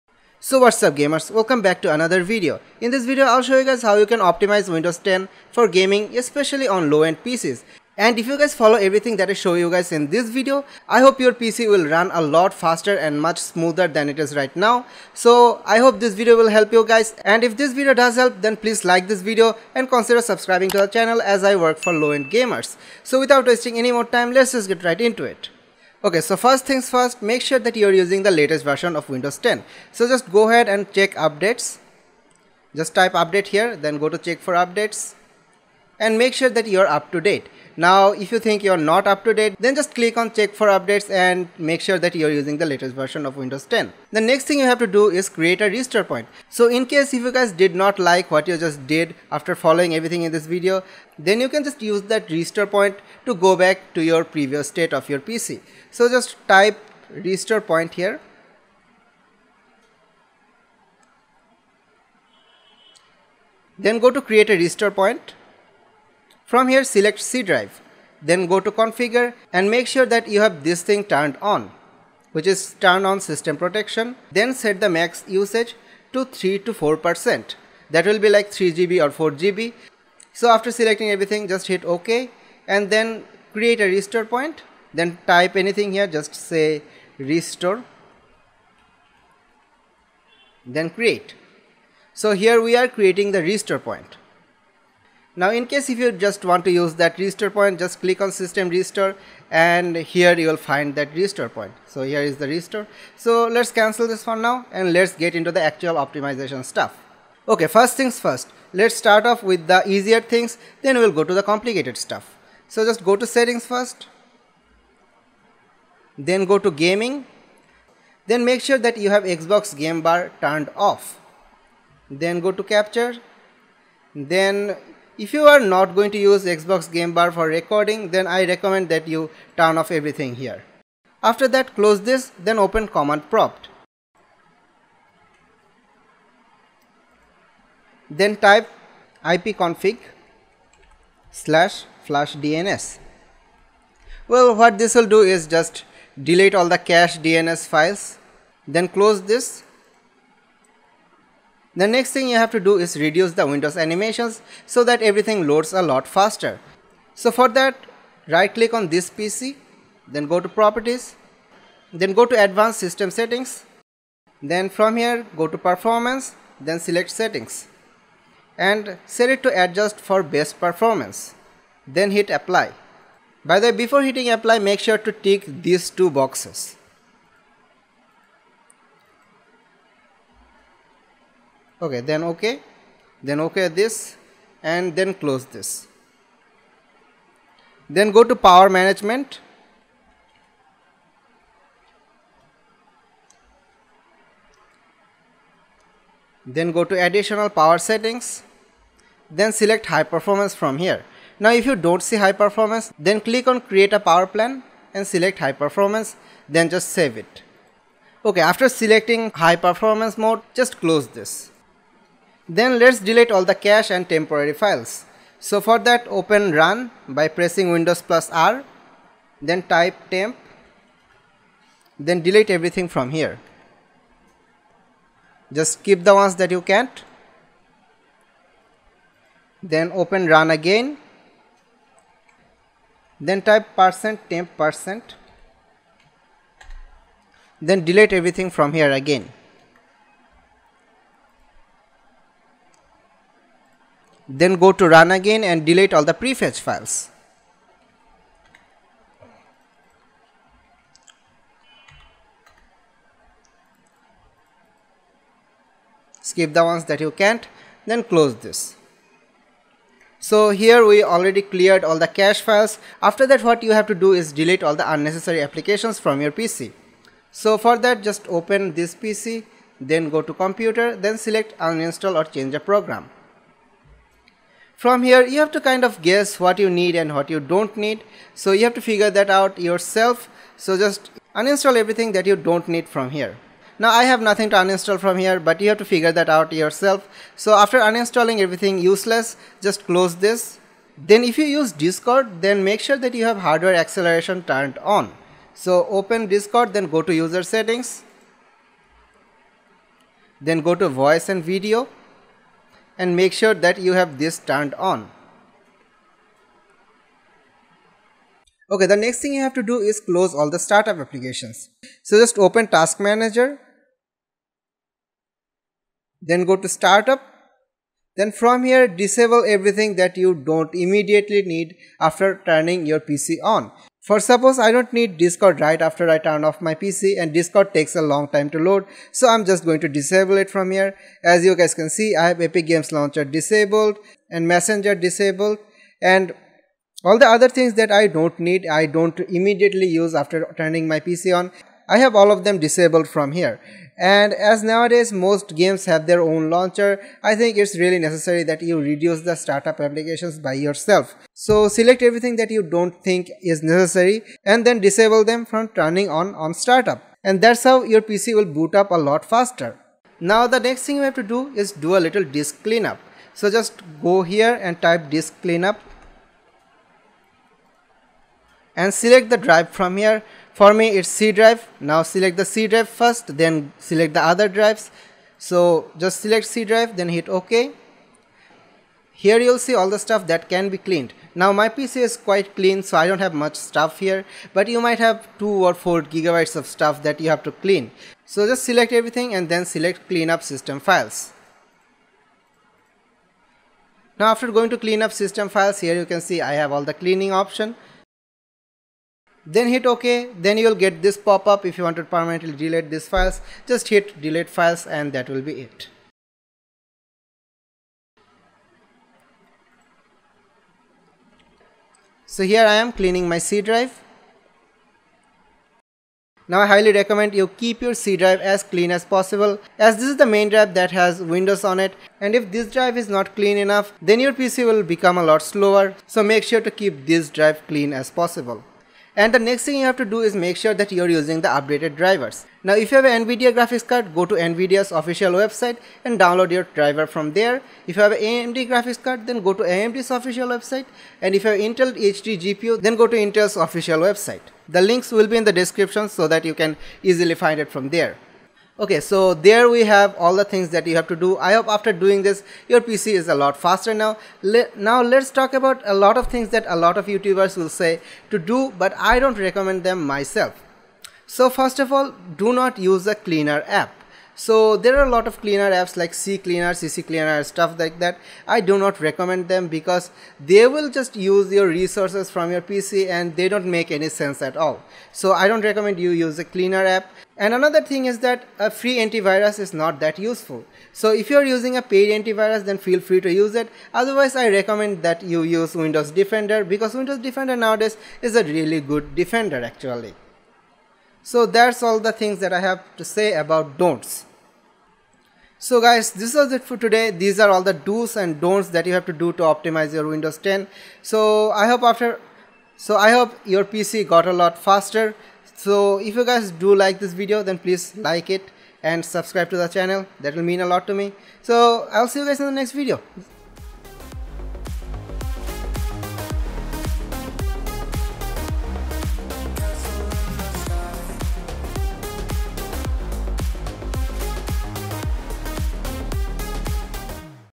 How To Optimize Windows 10 For GAMING For LOW END PC !!! Make Your PC Faster By 200%!!!!
Music Credits 🎵 🎵 🎵: N/A

Check ‘About’ section to know more about me.